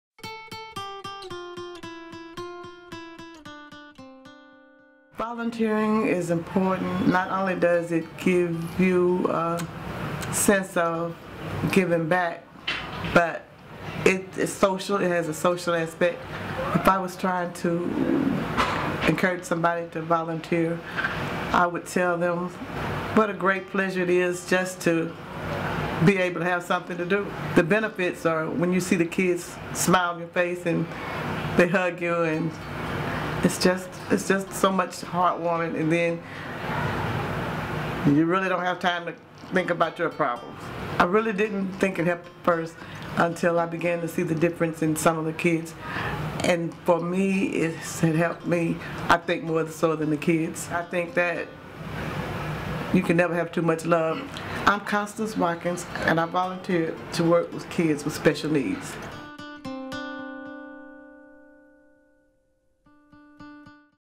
Youth Program and Education Volunteer of the Year for 2013
Constance Watkins, a Foster Grandparent, won for her work as an education assistant at Siskin Institute for Children, where she cares for the children, provides a loving environment, reads to them and engages them.